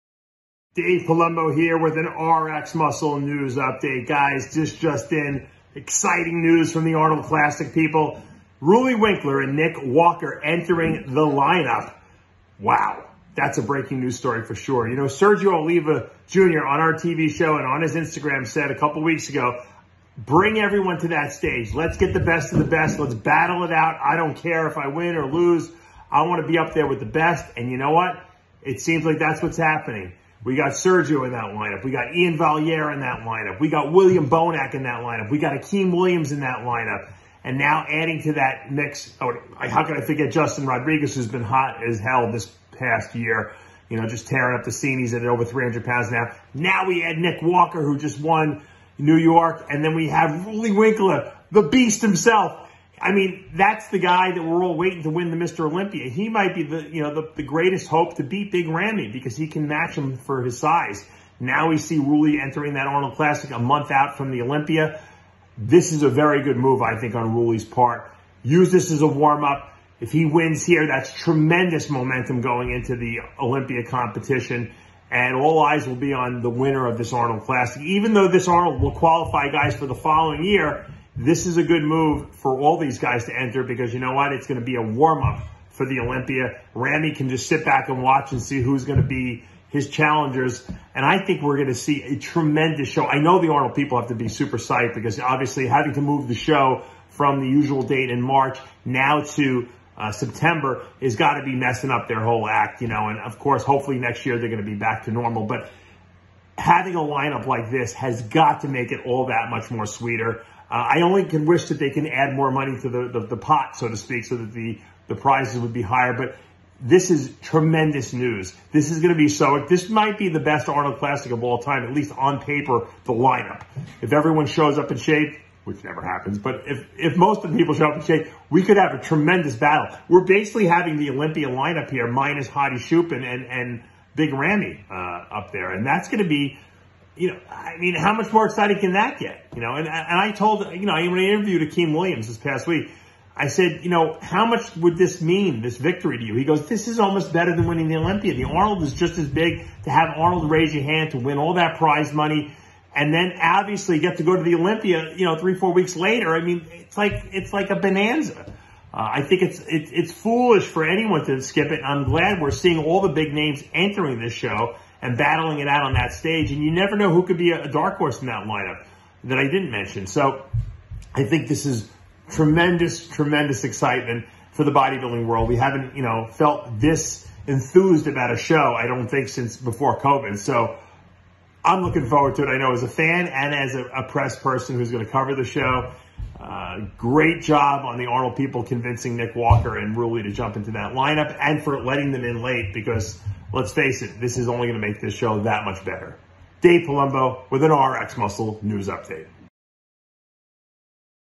NICK WALKER & ROELLY WINKLAAR ADDED TO ARNOLD LINEUP!
Dave Palumbo reacts to the announcement from the Arnold Classic that perennial Olympia title threat Roelly Winklaar and '21 New York Pro champion Nick Walker have both been added to the 2021 Arnold Classic line-up that already includes the likes of defending champion William Bonac, Sergio Oliva Jr, 2017 champion Cedric McMillan, 2020 Olympia top-6 Akim Williams, 2020 top-7 Olympian Iain Valliere, '21 Indy Pro champ Justin Rodriguez, Hassan Mostafa, Steve Kuclo, Maxx Charles, Lionel Beyeke, and other top bodybuilding stars.